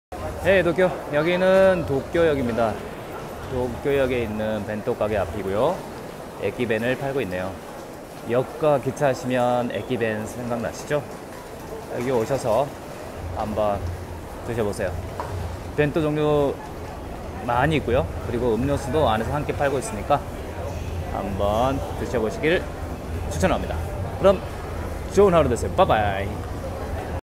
東京駅散歩. 도쿄역 산책. 駅弁 에키벤, 도시락, 도쿄여행 일본여행
東京駅散歩. 도쿄역 산책. 駅弁 에키벤
友達と一緒に歩きたい道。
친구와 함께 걷고 싶은 길.
Walk around Tokyo.

Tokyo Trip
東京散歩 도쿄산책
東京旅行 도쿄여행
日本旅行 일본여행